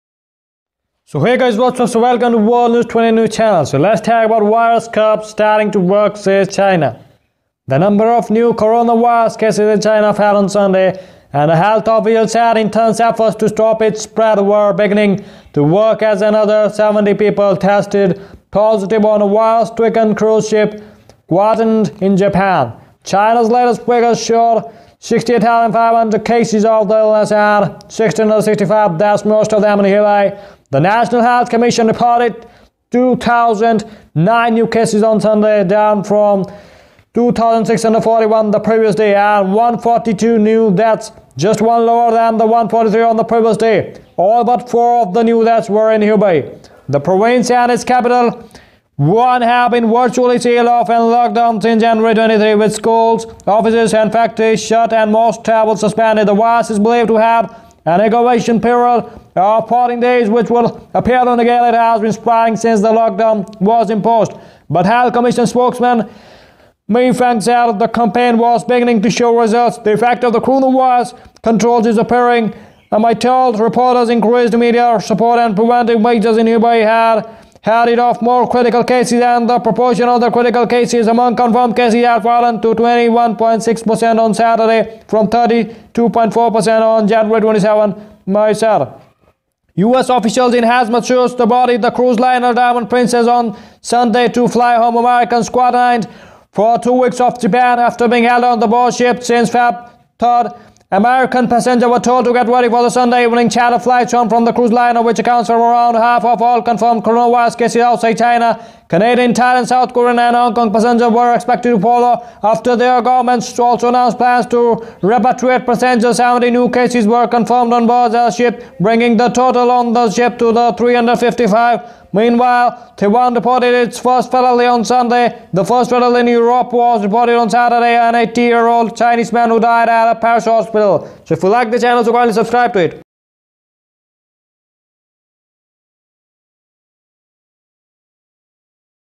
CORONAVIRUS TOLL RISES TO 1,665 IN CHINA
The National Health Commission reported 2,009 new cases on Sunday, down from 2,641 the previous day, and 142 new deaths, just one lower than the 143 on the previous day.